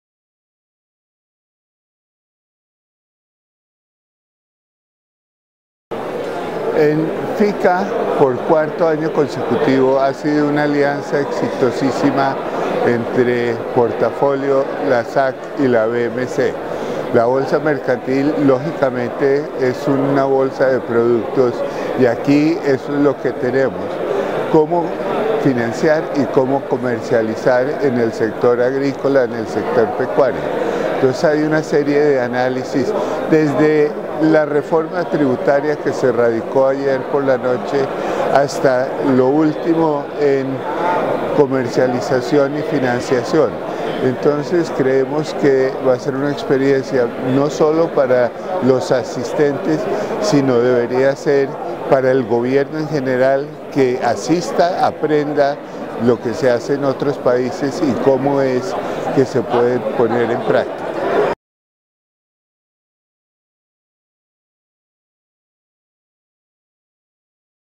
La importancia del IV Congreso de Financiamiento y Comercialización Agropecuaria, FYCA 2016
Entrevista a Rafael Mejía Presidente Bolsa Mercantil de Colombia​